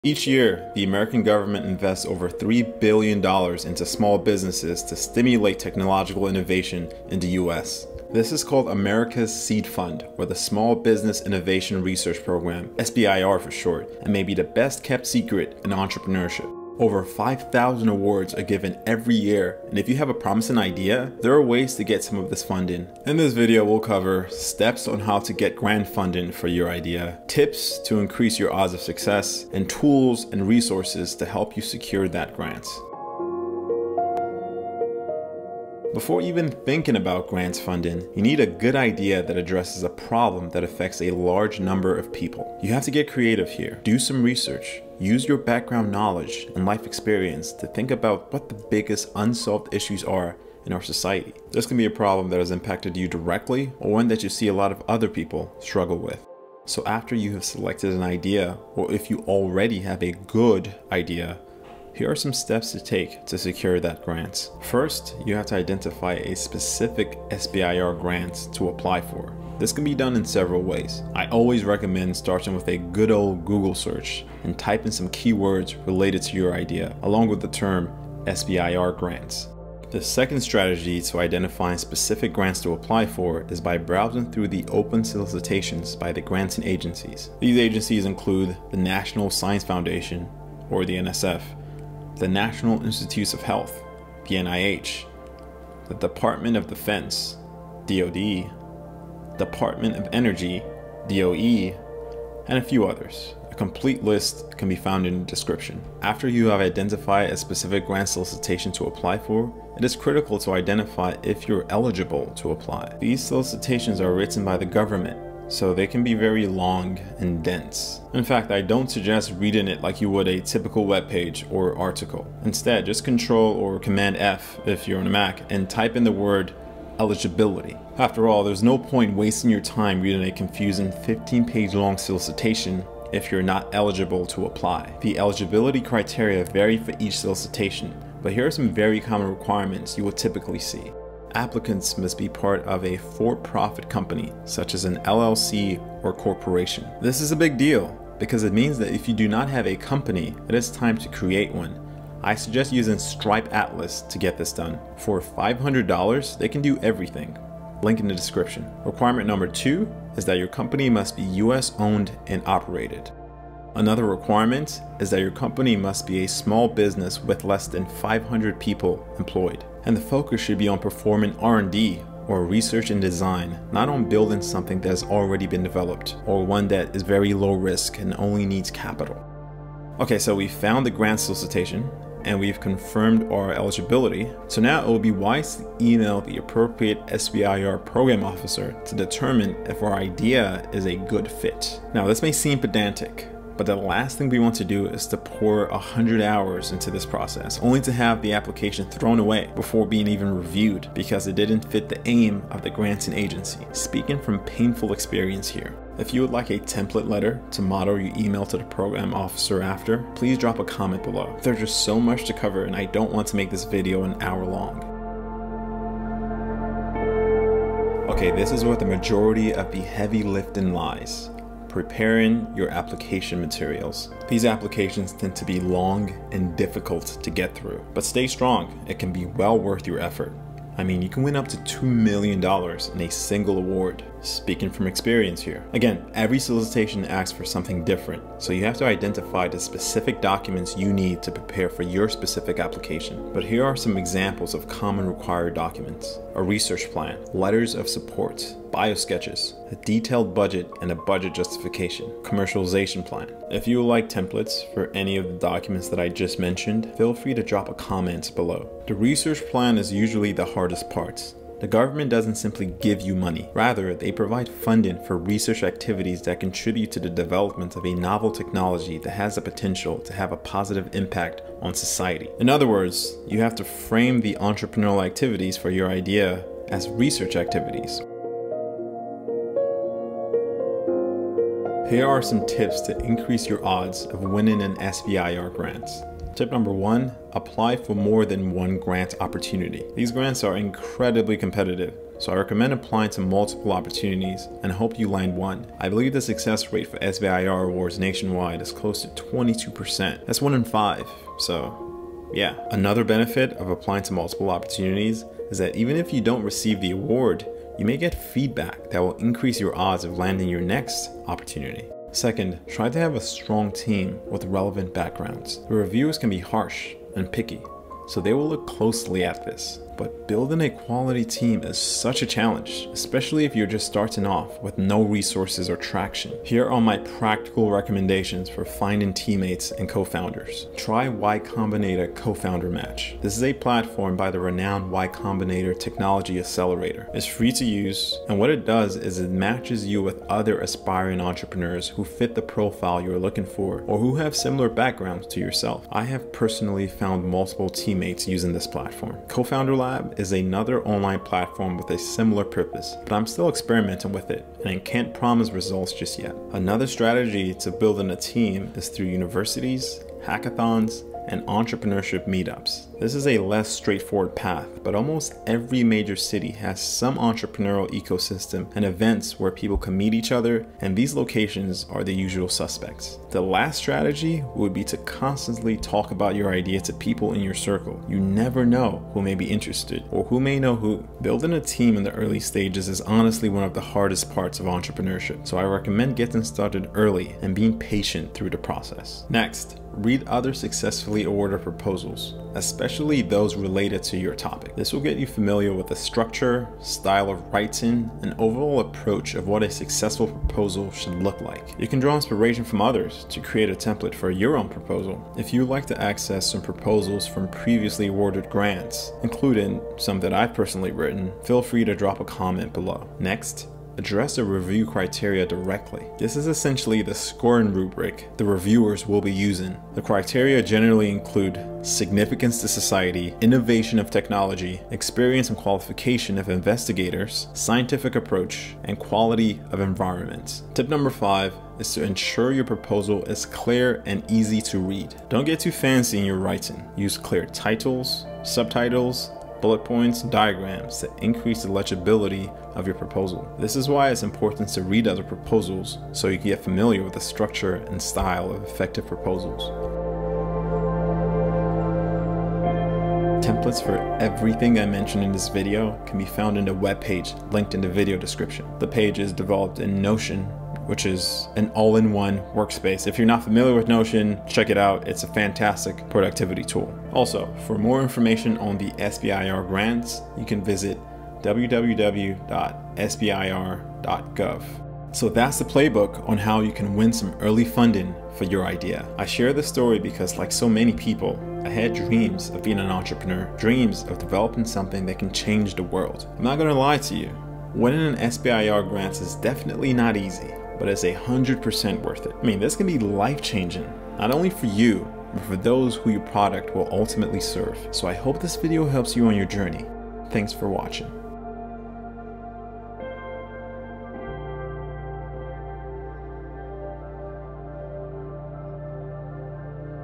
How to get Startup Funding - SBIR | America’s Seed Fund
A guide to gaining funding for your startup ideas through the SBIR grant program, America’s Seed Fund. As a recipient of two SBIR grant from both the NIH and NSF, I wanted to share my experience with aspiring entrepreneurs and dreamers!

Institutions such as the National Science Foundation (NSF) and the National Institutes of Health (NIH) give billions of dollars to smallbusinesses working on innovative ideas that can improve society.

In this video, we will explore:
00:34 | Steps to get grant funding for your idea
04:31 | Tips to increase odds of success
07:32 | Tools and resources guide you along the journey